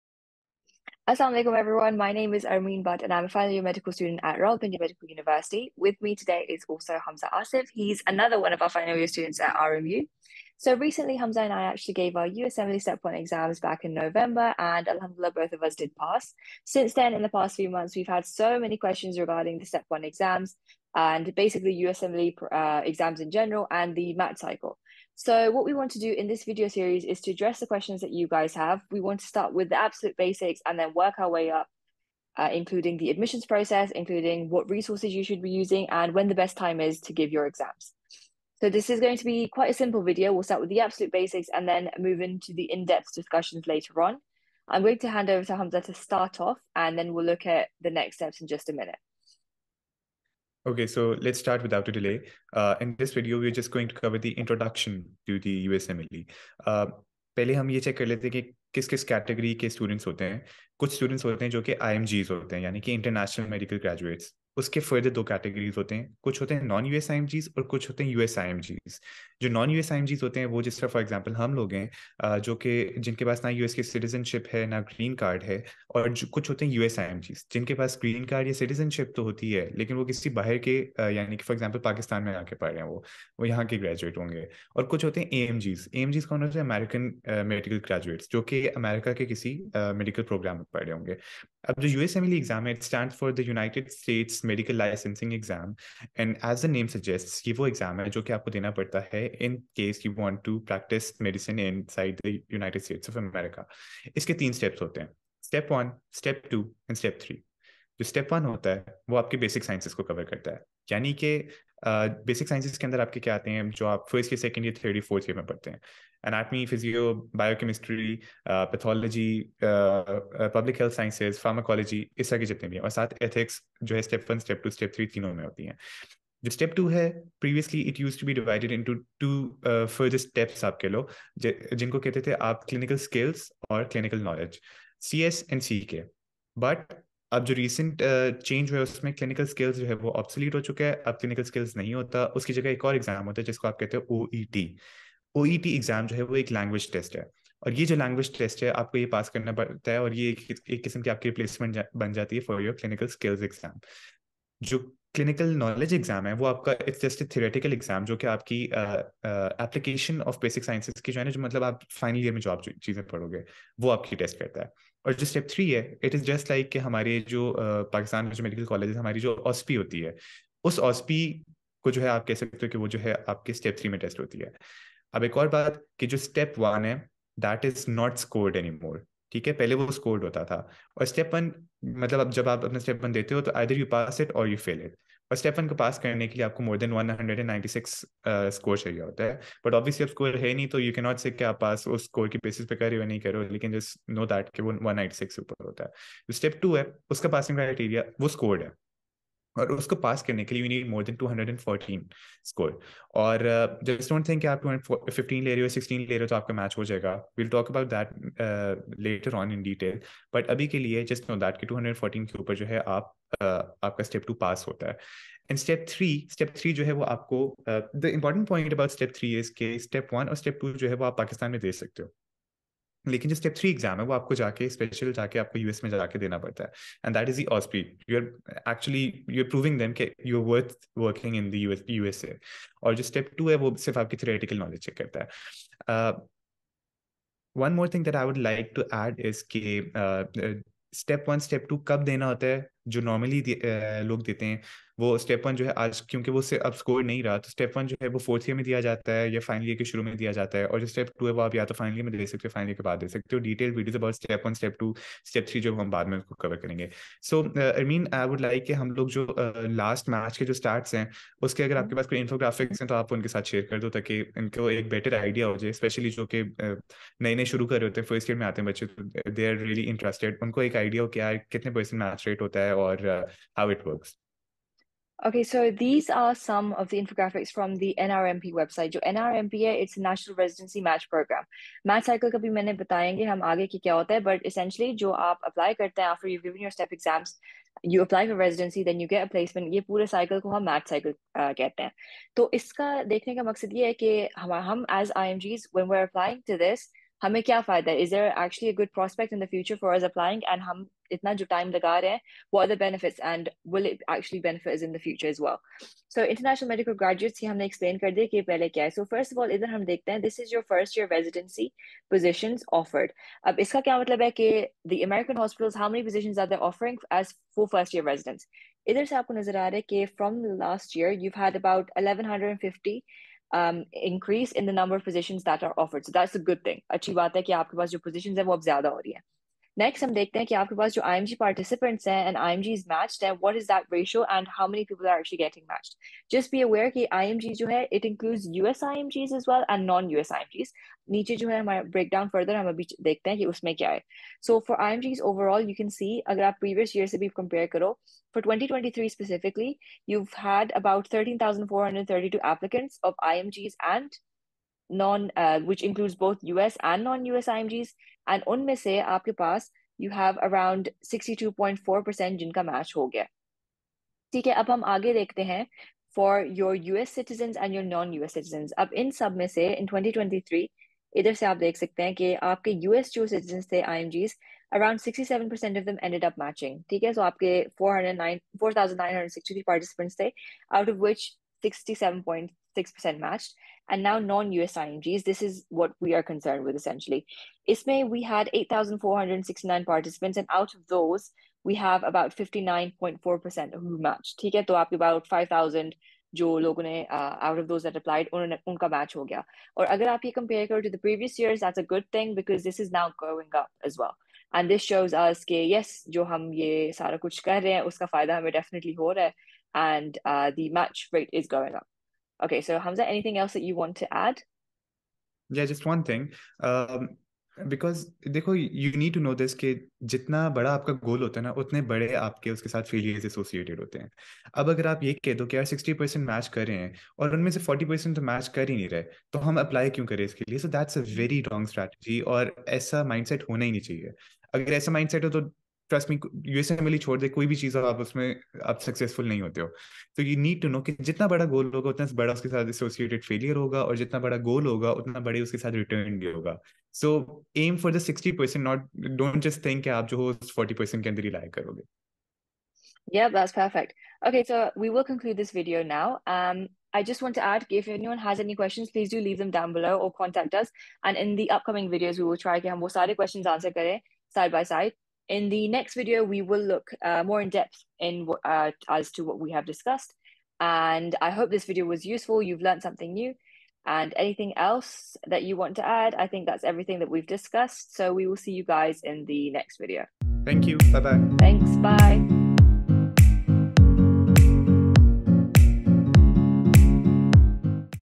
USMLE from the Basics + Authentic ECFMG Match Statistics for IMGs (2022/23) | ScrubbedIn
This video series is meant to guide medical students of Pakistan in the most basic way, so every student from the First to Final Year MBBS can understand it.

This video highlights what the USMLE is, the difference between AMGs and IMGs (US & Non-US), the latest update on Step 2 CS, and the authentic statistics for IMGs issued by the ECFMG for the last match (2022/2023).

CHAPTERS
00:00 Channel Introduction
00:56 AMG vs IMG (US and Non-US)
01:30 USMLE Introduction
04:30 ECFMG Match Statistics (2022/23)

Hamza's IG: @hamza_asif51
Armeen's IG: @armeen.butt